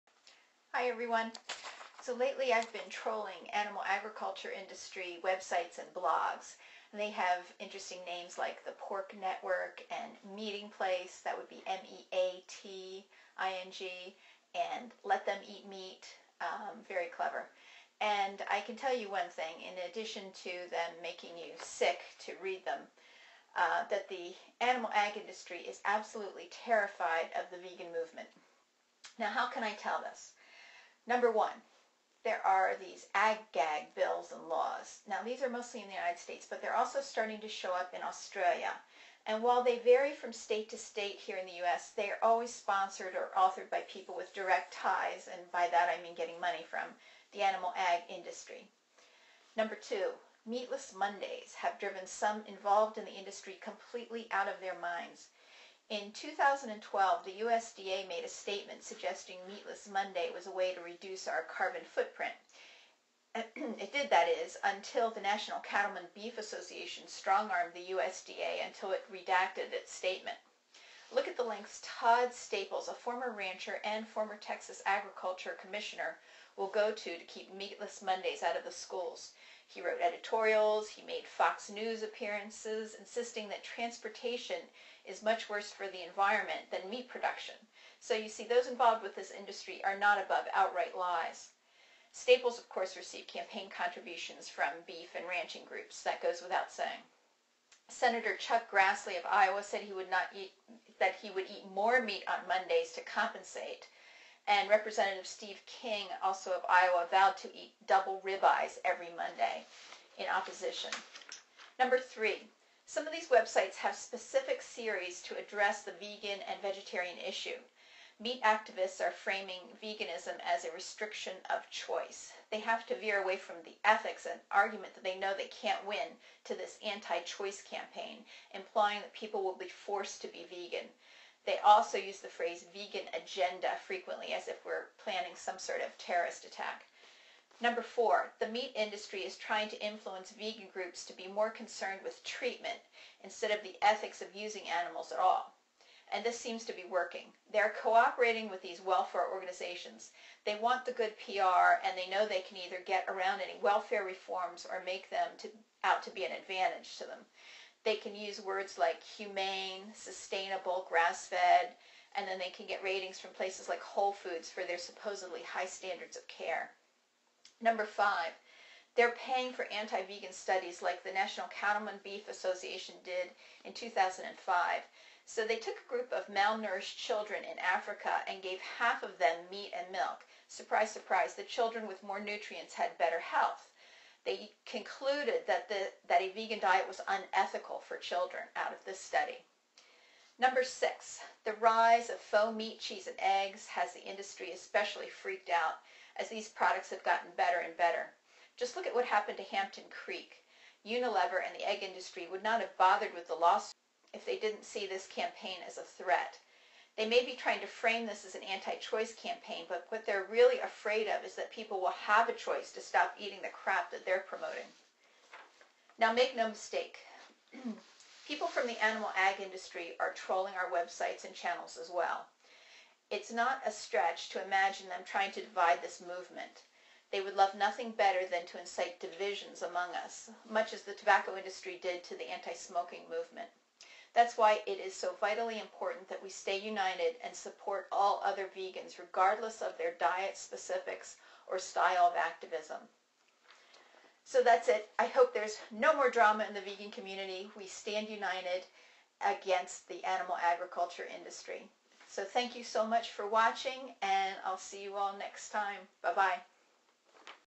They're Very Afraid of Us
For more information on the efforts of the Big Food industry to copy the tactics of the tobacco industry used against the anti-smoking movement see the link to Dr. Greger's video below.